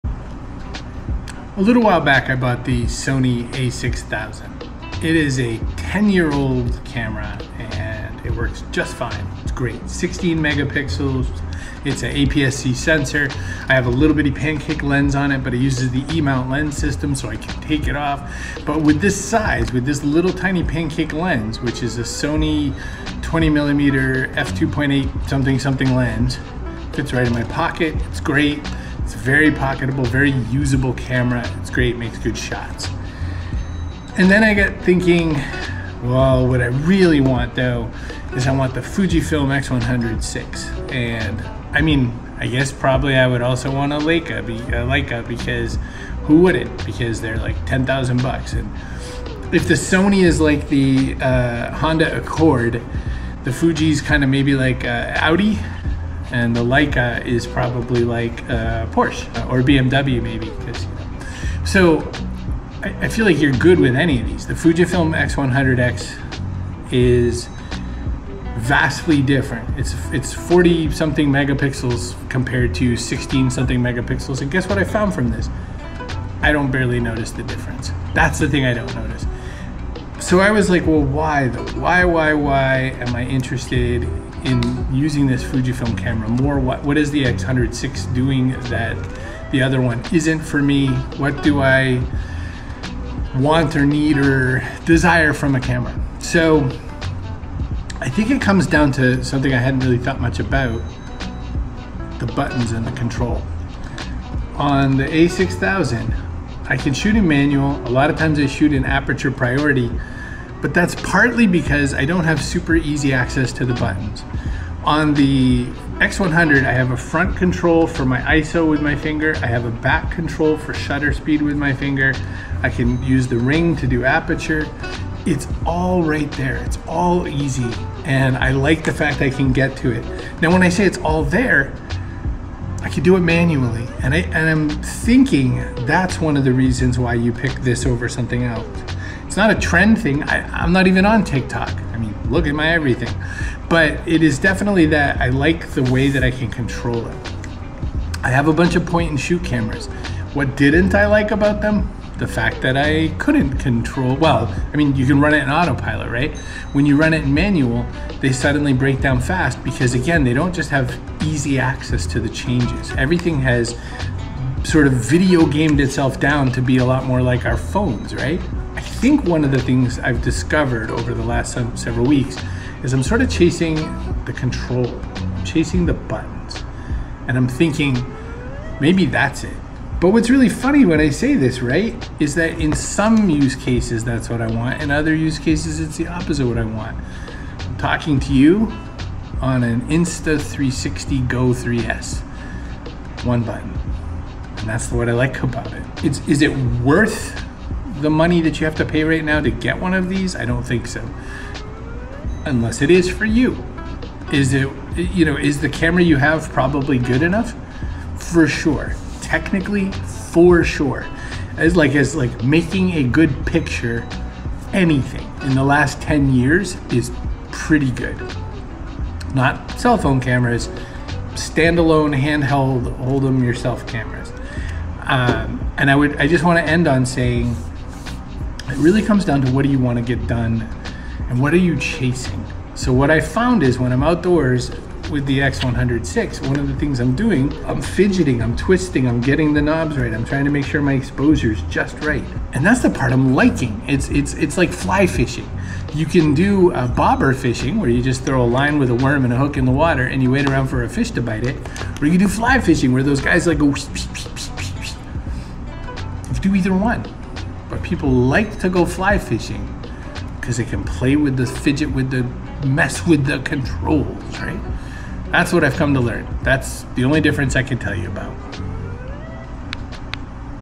Is the Fujifilm x100vi Worth It? - Not a Review - A Reflection
I got the Fujifilm x100vi and immediately started wondering WHY I did. I was perfectly happy with my point and shoots, happy with the Sony A6000 I picked up. So what was it?

Sure wasn't TikTok. I'm too old to even use that app. So what got me hooked? What has me fidgeting and twisting and poking when I could just as easily point and shoot? Watch to find out.